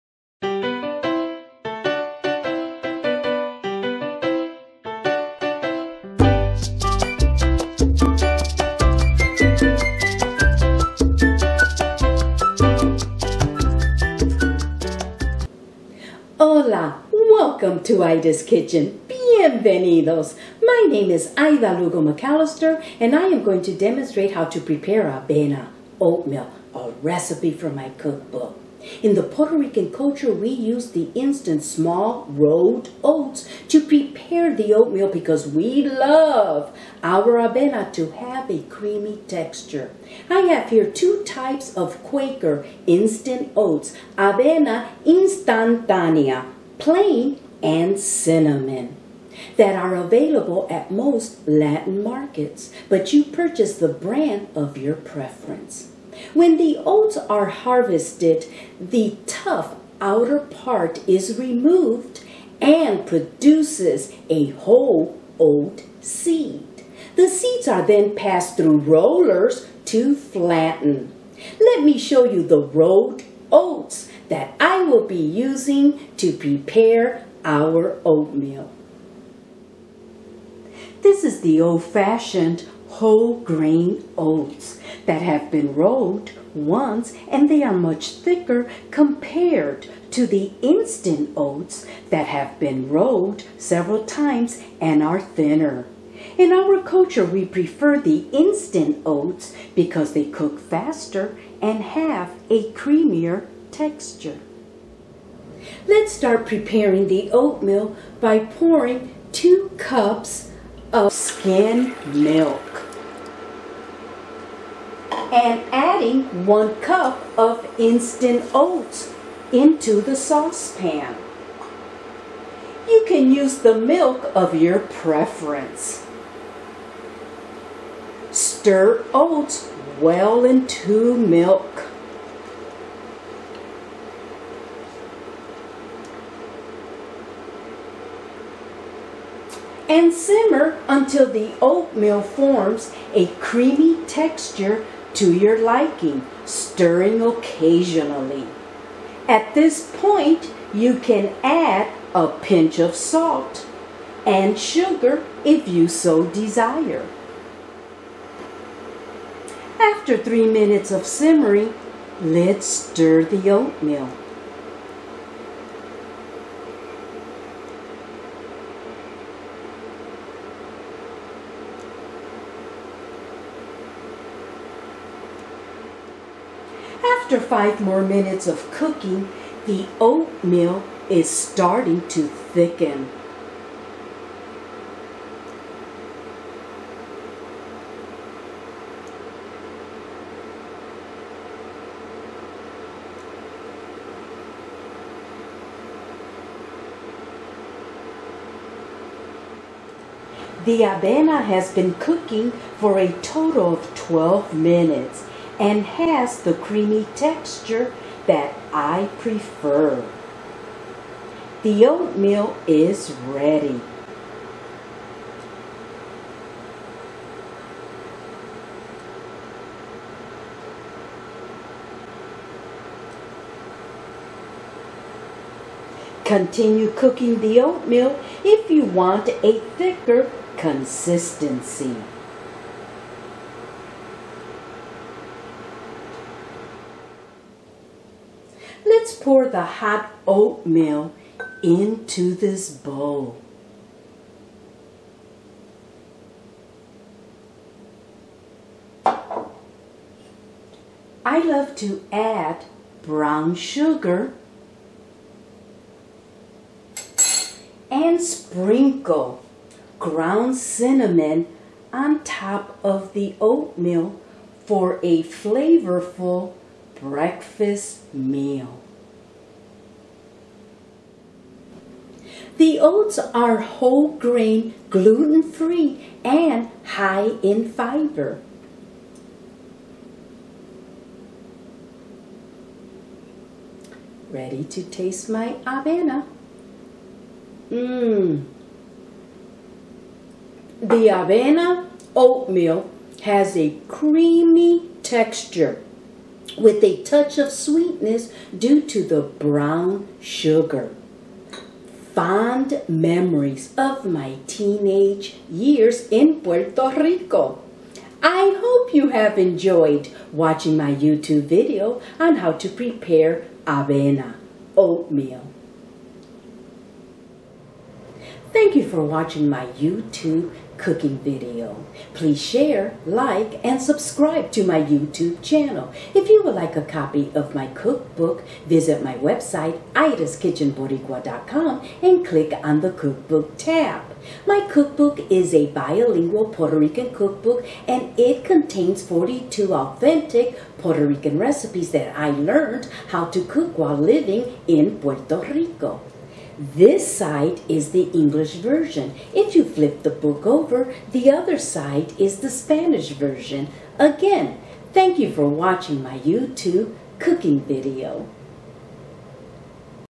Avena (Oatmeal)
An updated version.  This creamy avena (oatmeal) is prepared with instant oats and flavored with brown sugar and ground cinnamon for a fulfilling and healthy breakfast meal.